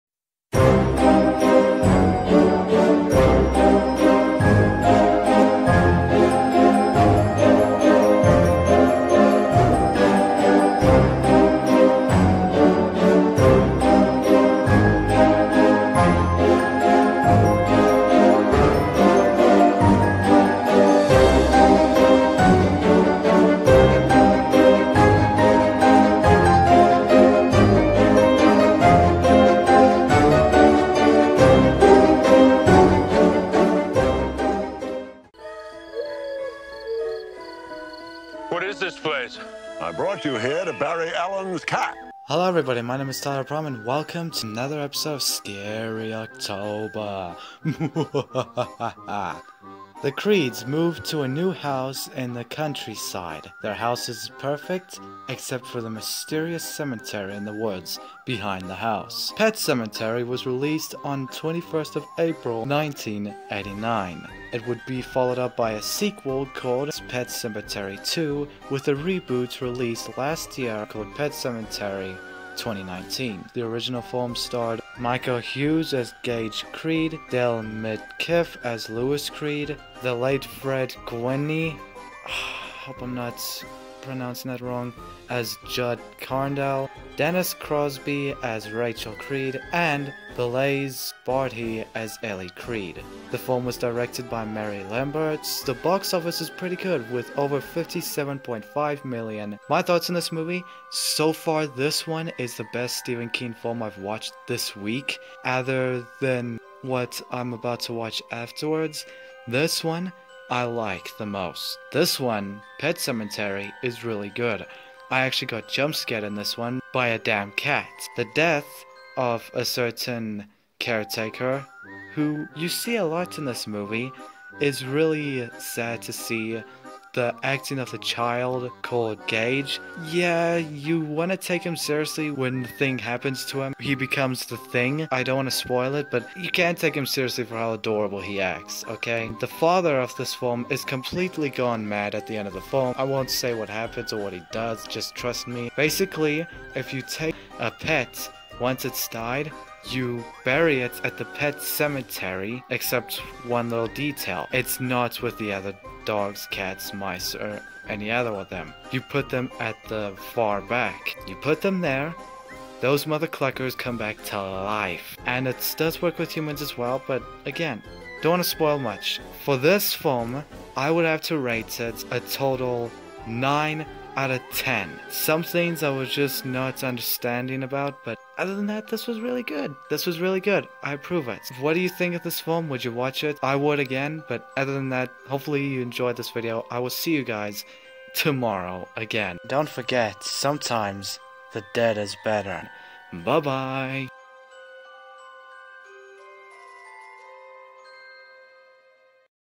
Pet Sematary - Movie Review (Stephen King Week]
In today's episode of Scary October, we find out... that sometimes, Dead is Better.